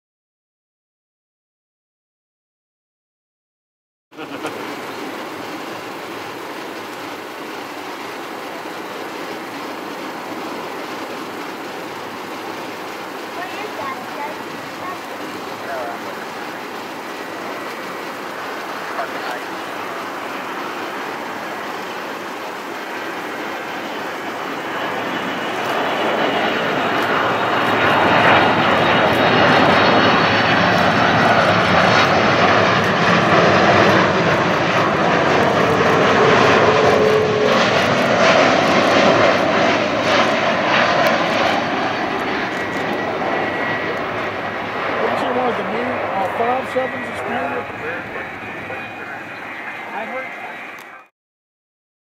American Airlines 767-323/ER [N389AA]
American Airlines 767-323/ER landing on 18R at Dallas/Fort Worth International Airport (DFW) on September 20, 2014